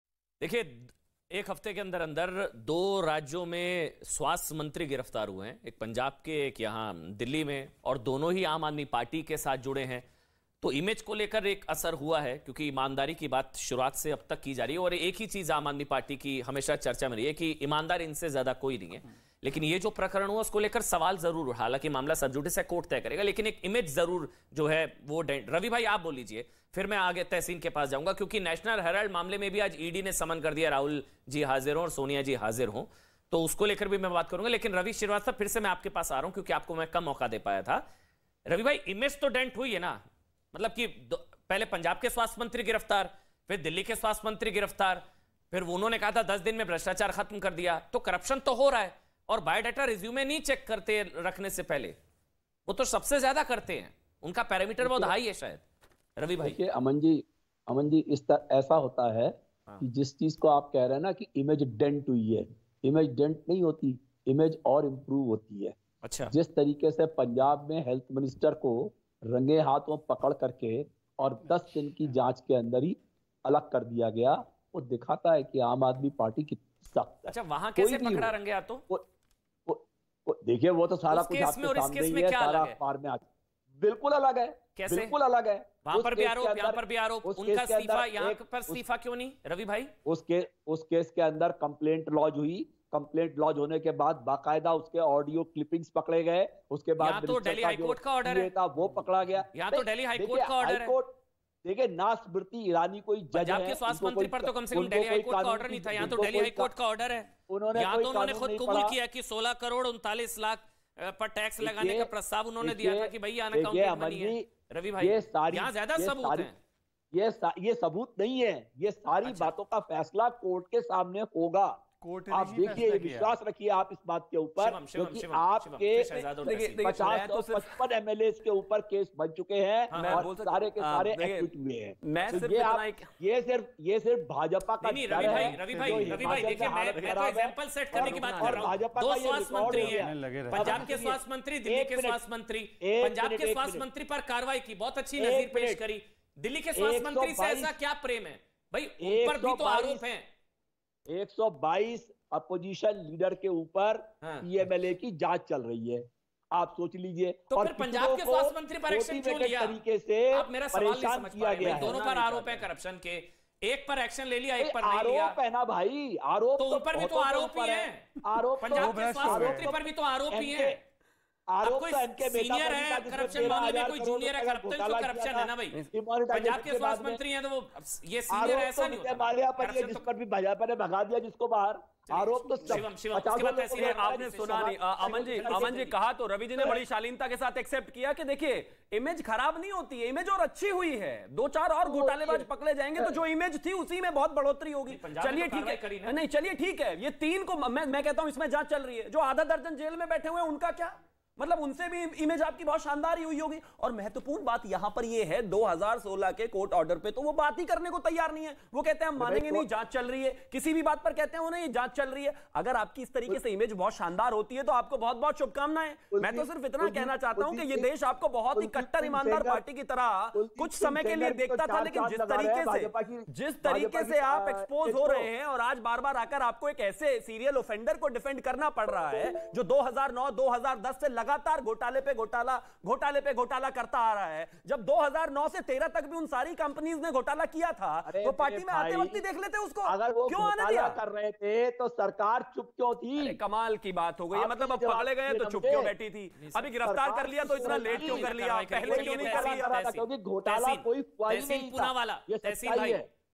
Punjab और Delhi के स्वास्थ्य मंत्री दोनों एक हफ्ते में गिरफ्तार? देखें क्या बोले रवि श्रीवास्तव
रवि श्रीवास्तव ने कहा कि आम आदमी पार्टी की कोई इमेज खराब नहीं हुई है बल्कि इमेज और अच्छी हुई है. रही बात आरोप कि तो कोर्ट में तय होगा. ये सब भाजपा का डर है क्योंकि आरोप तो सैंकड़ों लोगों पर है.